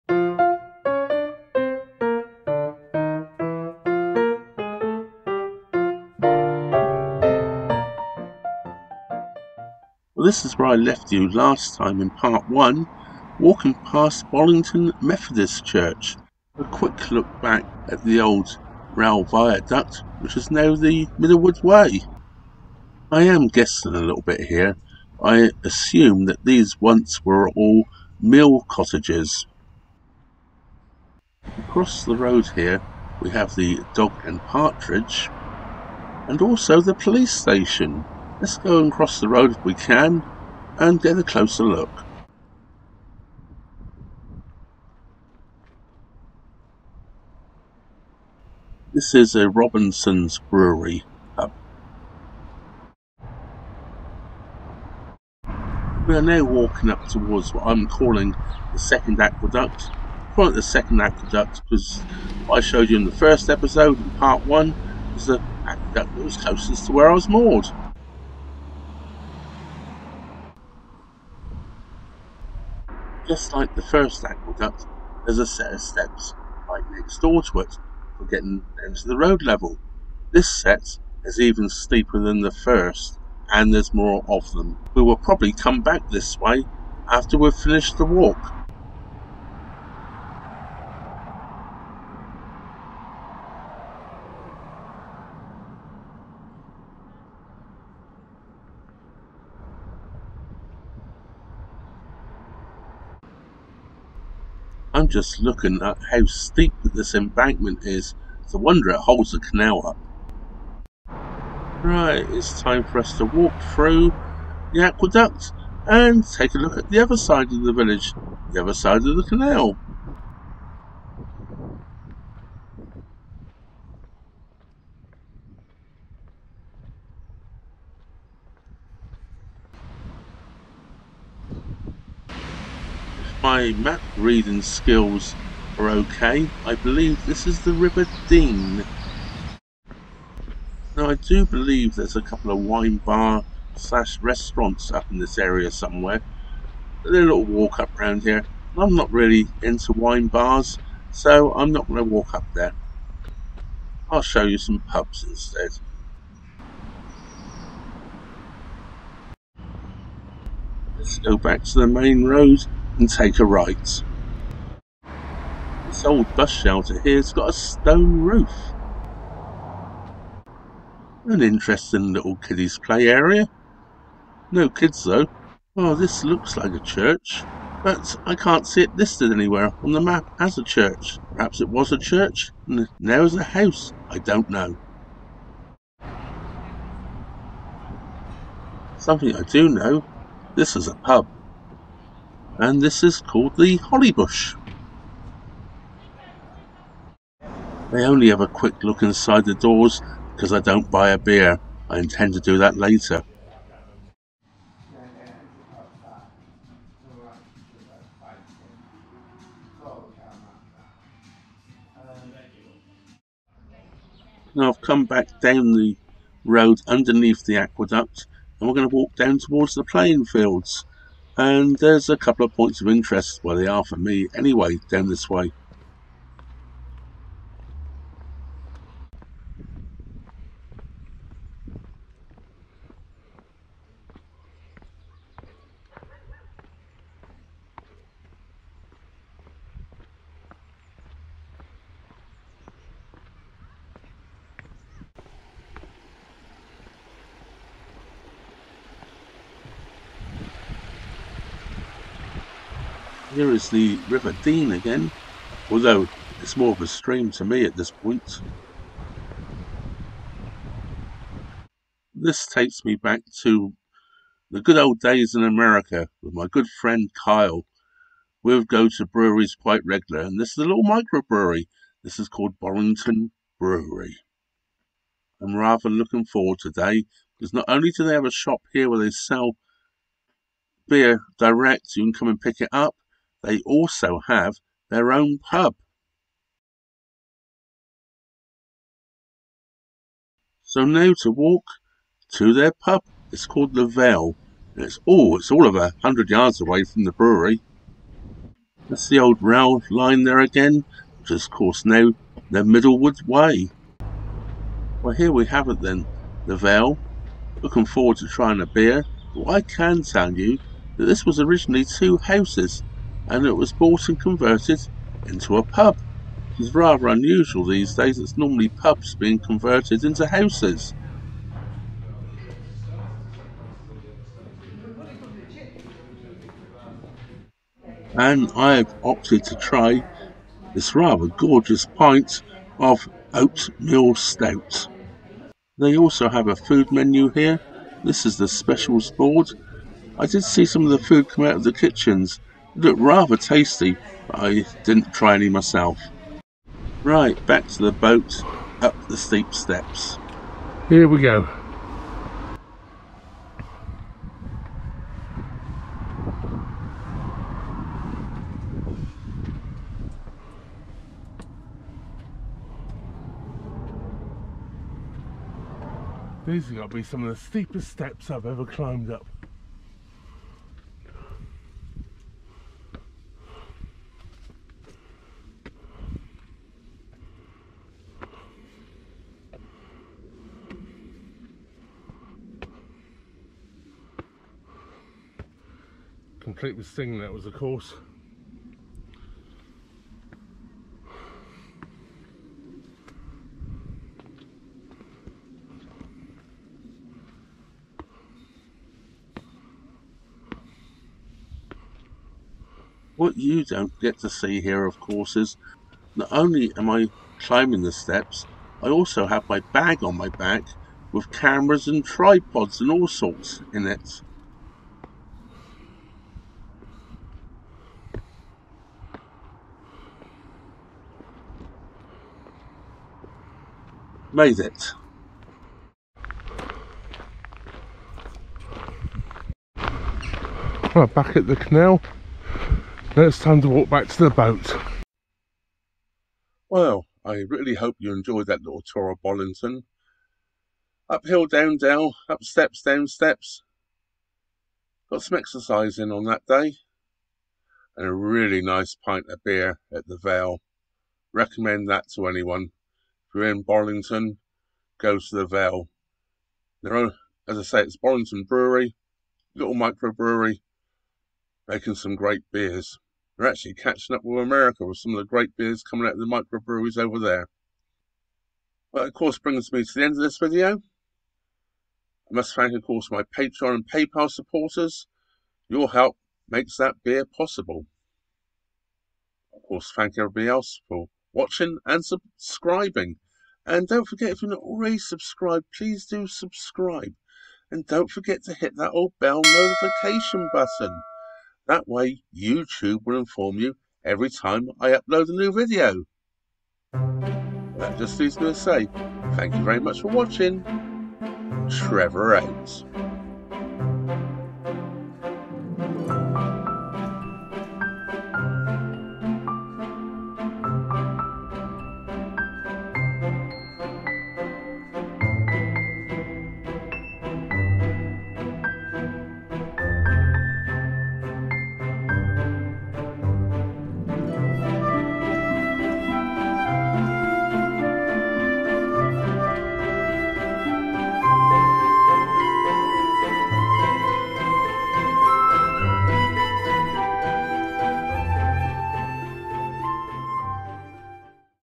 Bollington Part 2 - "Off The Cut"
Being moored up outside the Adelphi Silk Mill, I had to take a walk into the Beautiful village of Bollington. Join me on my walk, I'll show you the shops, churches, pubs and any other places I think is of interest. Part 2.